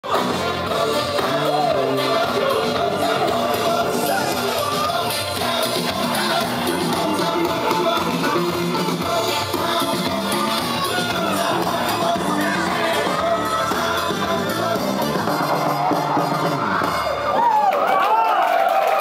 Barrio Latino Baku - Black & White party v3 18.01.2015
Barrio Latino Baku - Black & White party with Tofig Aliyev, Alimardan Aliyev, Farid Mehdiyev and etc...